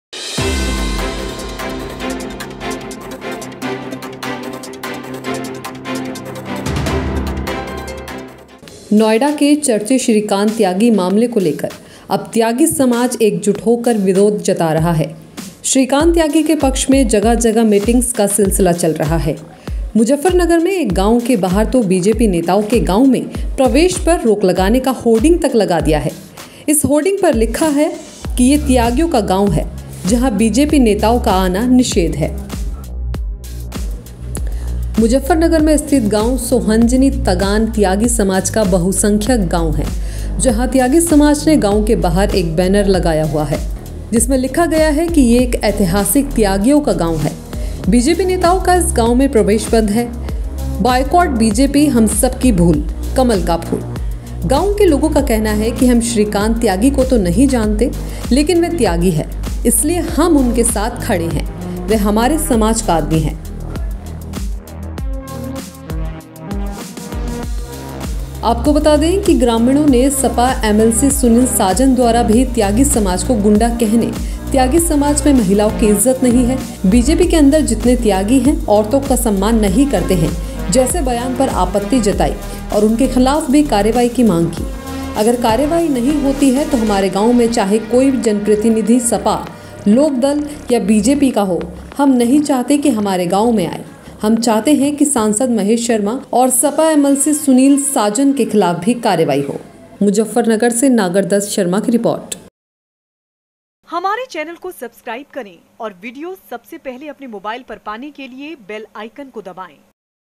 SHRIKANT TYAGI: के समर्थन में होर्डिंग, लिखा- 'त्यागियों का गांव है बीजेपी नेताओं का आना मना
श्रीकांत त्यागी के समर्थन में मुजफ्फरनगर के गांव के बाहर एक होर्डिंग लगा दी गई है जिस पर लिखा है "ये त्यागियों का गांव हैं जहां बीजेपी नेताओं का आना निषेध है."
UP का सबसे भरोसेमंद न्यूज़ नेटवर्क
Twitter पर Follow करें @ApnaUpradesh